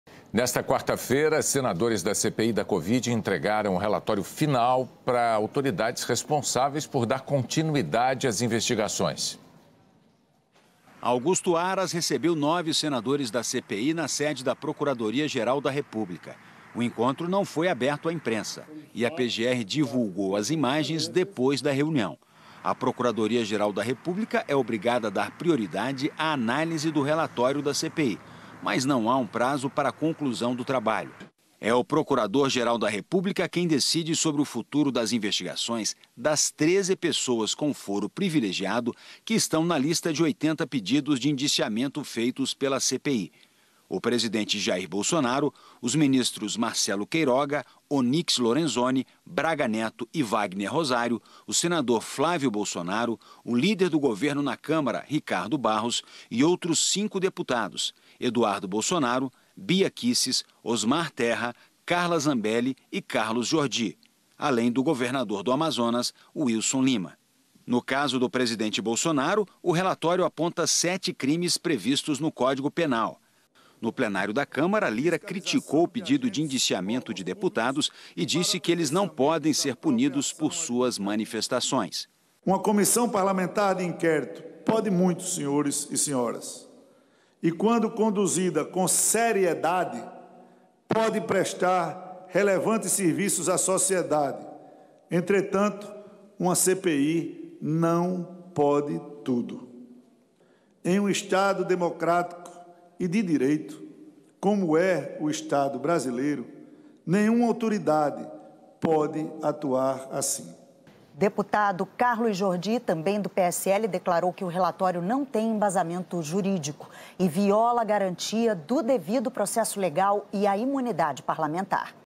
O RELATÓRIO É NULO, VIOLA A GARANTIA DO DEVIDO PROCESSO LEGAL E A IMUNIDADE MATERIAL PARLAMENTAR
O relatório é nulo, não possui embasamento jurídico, viola a garantia do devido processo legal e a imunidade material parlamentar. O jornal nacional se “esqueceu” apenas de citar o final da minha nota em que digo que “o relatório já nasce engavetado”.